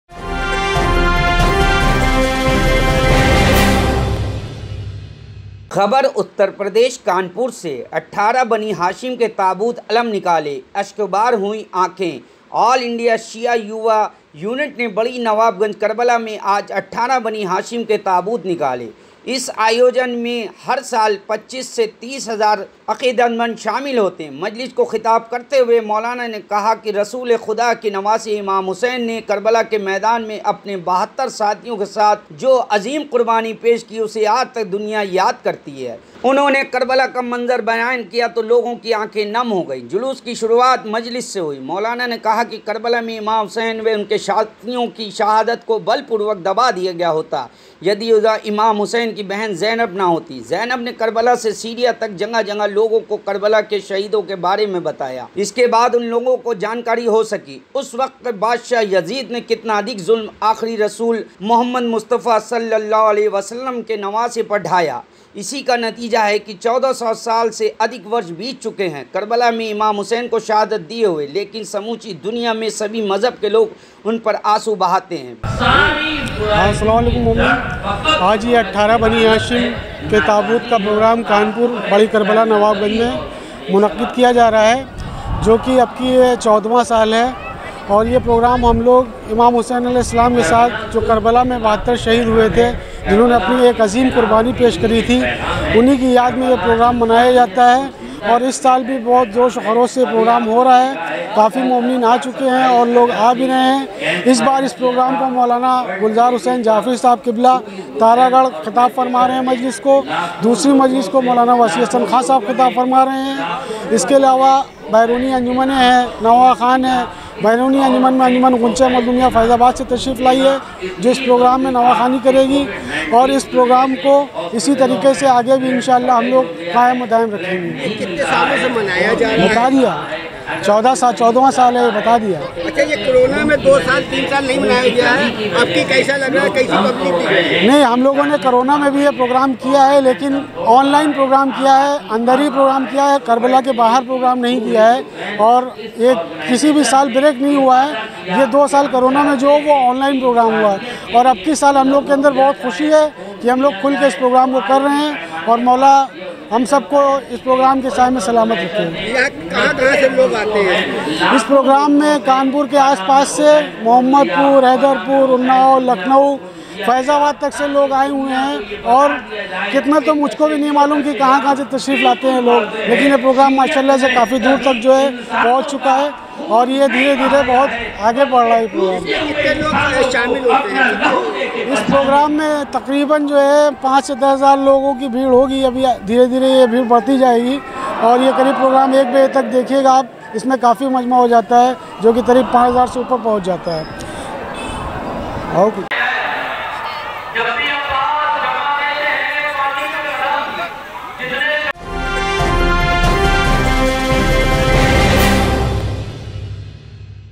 Kanpur : 18 बनी हाशिम के ताबूत अलम निकाले, अश्कबार हुईं आंखें | JK NEWS
-ऑल इंडिया शिया युवा यूनिट ने बड़ी नवाबगंज में आज 18बनी हाशिम के ताबूत निकाले। इस आयोजन में हर वर्ष 25-30 हजार अकीदतमंद शामिल होते है   मजलिस को खिताब करते हुए मौलाना  कहा कि रसूले खुदा के नवासे इमाम हुसैन ने कर्बला के मैदान में अपने 72  साथियों के साथ जो अजीम कुर्बानी पेश की उसे आज तक दुनिया याद करती है। उन्होंने कर्बला का मंजर बयान किया तो लोगों की आंखें नम हो गईं।  जुलूस की शुरुआत मजलिस से हुई। मौलाना  ने  कहा कि कर्बला में इमाम हुसैन व उनके साथियों की शहादत को बलपूर्वक दबा दिया गया होता, यदि इमाम हुसैन की बहन जैनब न होतीं। जैनब ने कर्बला से सीरिया तक जगह जगह लोगों को कर्बला के शहीदों के बारे में बताया।इसके बाद लोगों को जानकारी हो सकी कि वक्त के बादशाह यजीद ने कितना अधिक जुल्म आखिरी रसूल मोहम्मद मुस्तफा अलैह के नवासे पर ढाया है। इसी का नतीजा है कि 1400 से अधिक वर्ष बीत चुके हैं कर्बला में इमाम हुसैन को शहादत दिए हुए, लेकिन समूची दुनिया में सभी मजहब के लोग उन पर आंसू बहाते हैं।